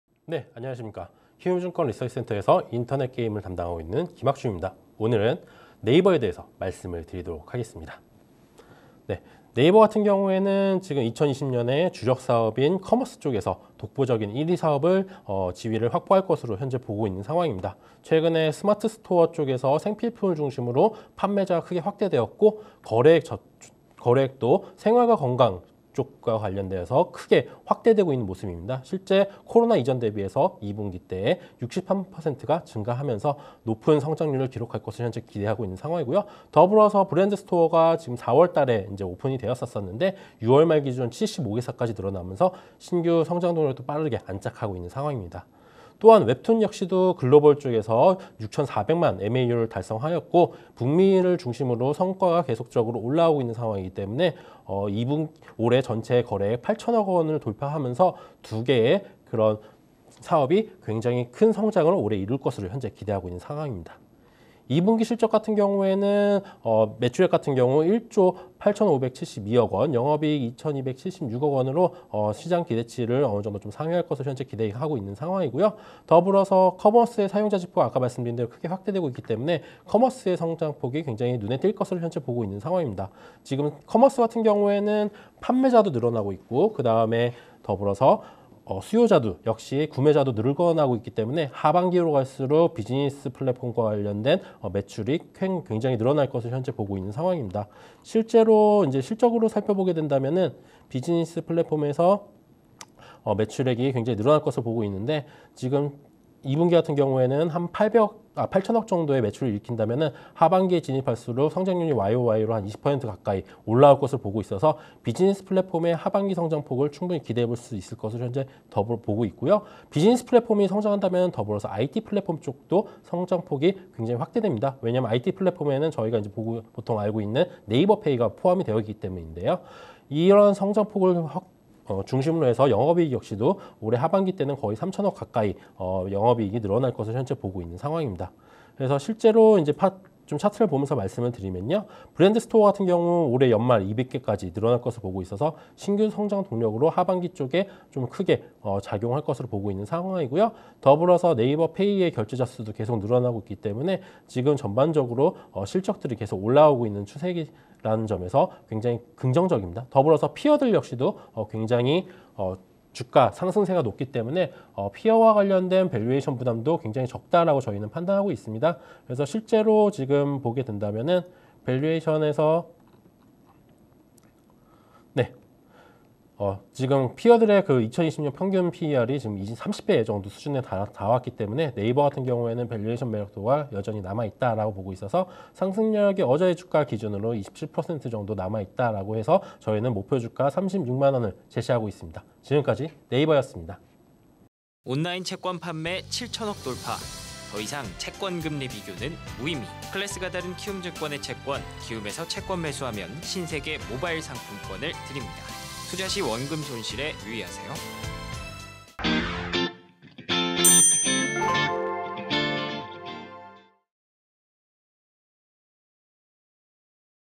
NAVER, 1황이 되려는 커머스와 북미웹툰 (애널리스트리포트.기업분석/20.07.09)
NAVER는 독보적인 커머스 성장과 웹툰의 확장성을 바탕으로
글로벌 Peer들 중에서 도 돋보이는 모습을 나타낼 것으로 기대
글로벌 전체적으로 플랫폼사들의 향후 성장성을 높게 평가하는 현 시점에서는
이에 딱 맞는 실적 성장을 보여줄 것으로 기대
2Q실적은 1조 8572억원(YoY +13.9%), 영업이익 2,276억원 (YoY +77.2%)을 기록하며
시장기대치를 충족할 것으로 전망, 관건은 쇼핑의 성장 폭
이번 코로나19를 기점 으로 커머스의 사용자지표가 크게 확대됨에 따라
하반기로 진입할수록 쇼핑 성장성은 더욱 커질 것으로 기대
웹툰의 확장성으로 매출성장 폭이 크겠지만 전체 콘텐츠 매출은 VLive의 콘서트 매출 회복세가
일부에 그칠 것으로 보여 작년 4Q 이상의 성과는 거두기 어려울 것으로 추정
 김학준 키움증권 기업분석팀 선임연구원

※해당 내용은 조사분석자료에 기반했으며 당사의 리서치센터가 신뢰할 수 있는 자료와 정보로부터 얻은 것이나 그 정확성이나 완전성을 보장할 수 없습니다.​